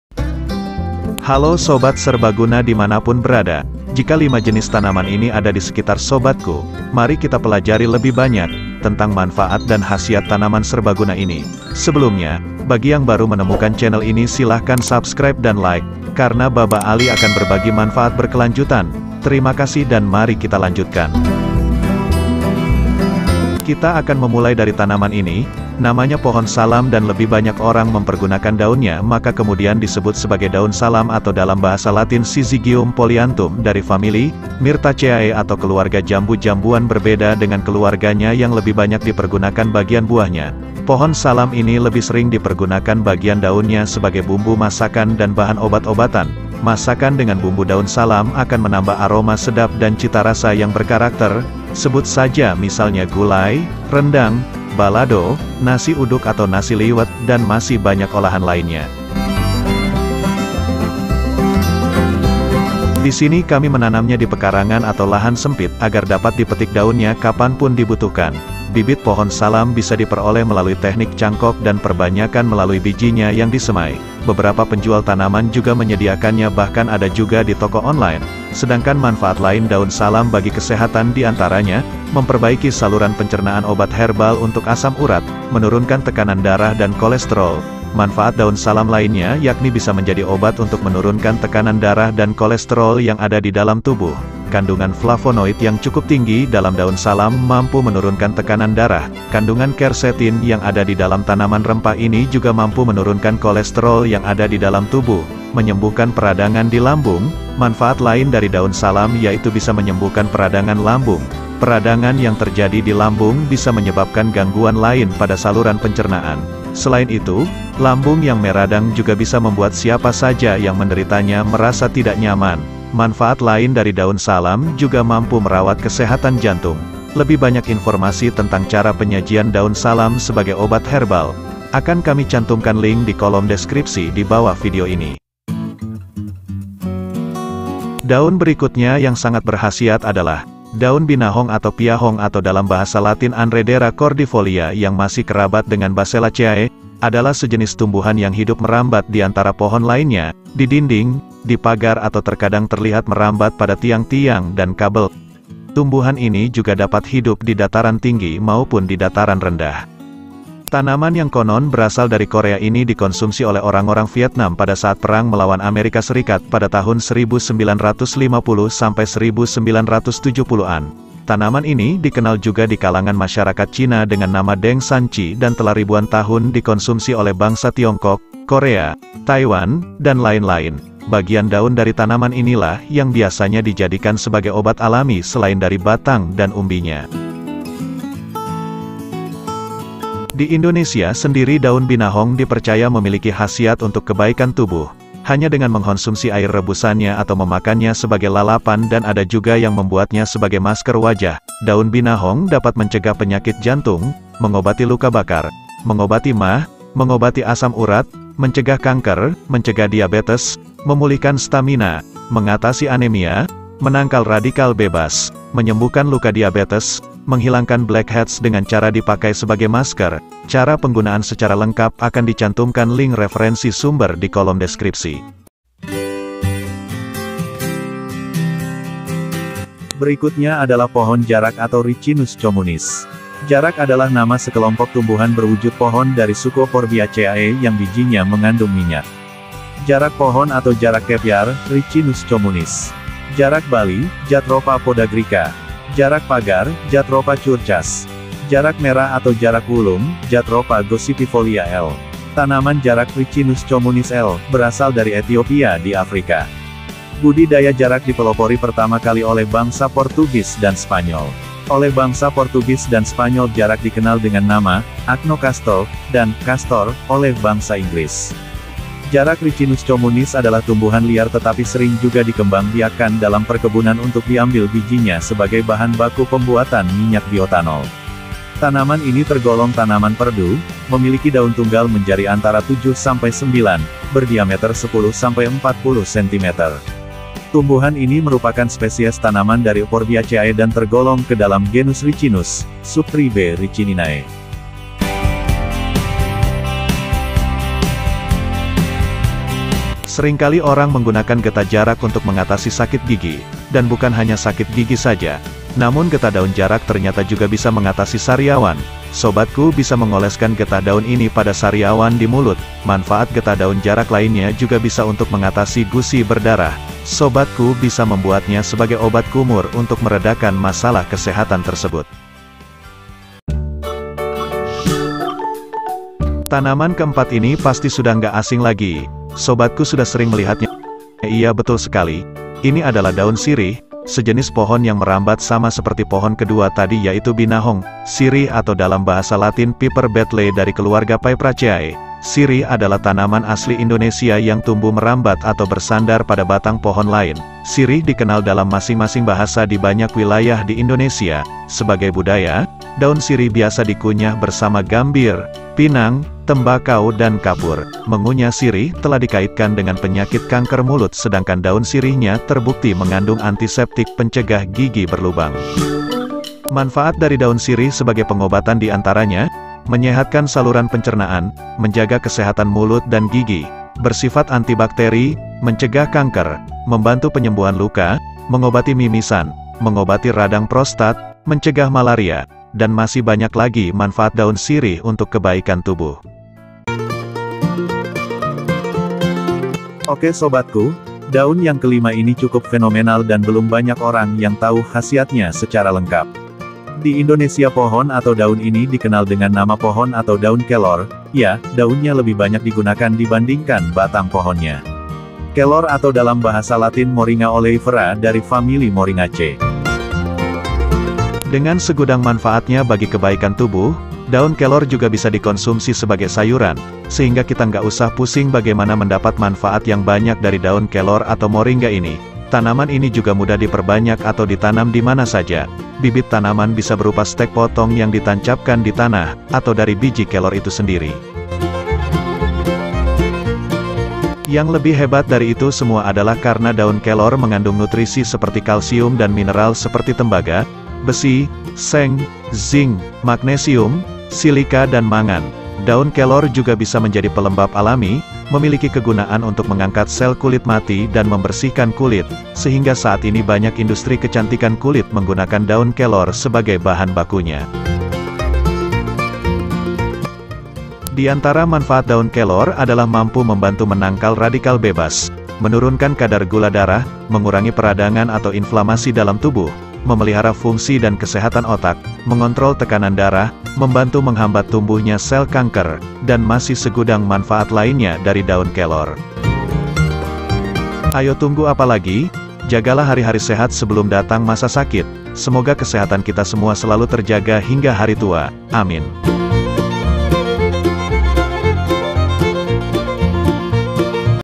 5 Jenis TANAMAN HERBAL Di Sekitar Kita tanamanobat #188
Karna mengetahui Manfaatnya, 5 jenis Tanaman Serbaguna ini sering di tanam di pekarangan sebagai Apotik hidup atau taman herbal, atau orang mengenal dengan istilah Tanaman Obat Keluarga ( TOGA ) agar dapat dipetik kapanpun ketika diperlukan. @HydrofarmTVIndo

5 Jenis Tanaman ini sudah lama diketahui Masyarakat, sangat berguna untuk perawatan kesehatan maupun untuk pengobatan tradisional yang alami. @DodiSatria